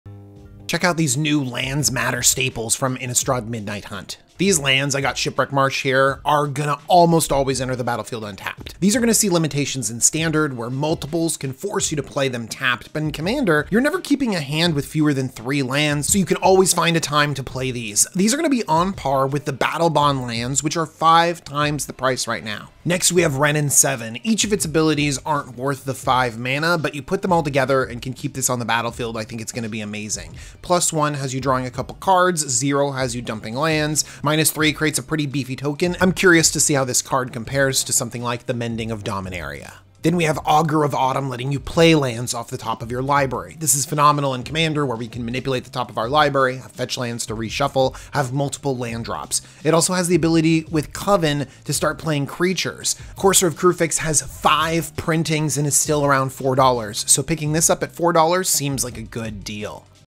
Commander LAND SYNERGIES is New Innistrad
This set has some very cool land synergies including some lands that will be invaluable.